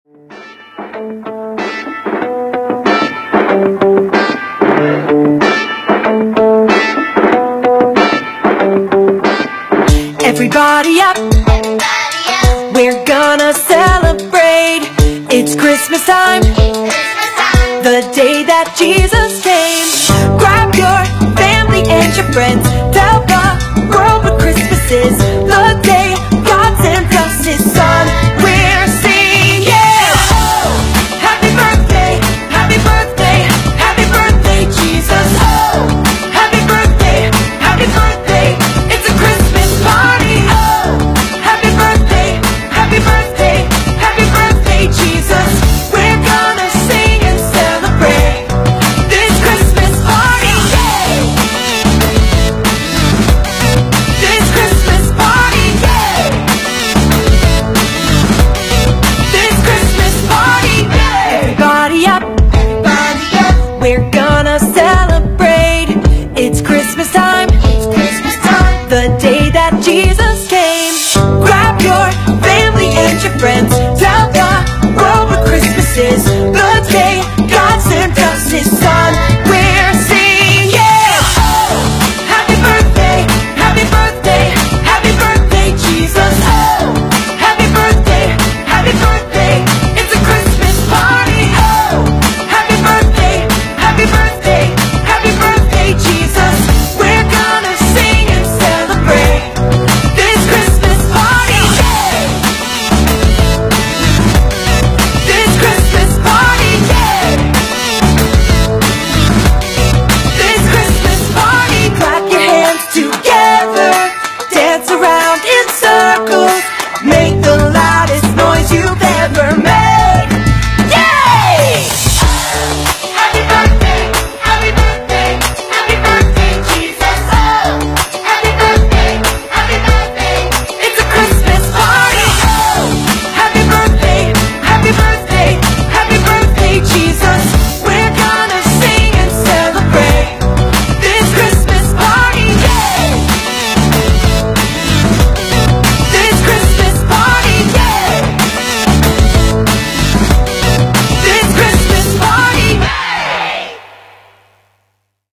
Christmas Party | Morning Star Academy | Pre school dance
Dance performed by students of Morning Star Academy,
Sion - bandra Link Road, Kalakilla, Dharavi, Mumbai.
May God be glorified through the act of these lil ones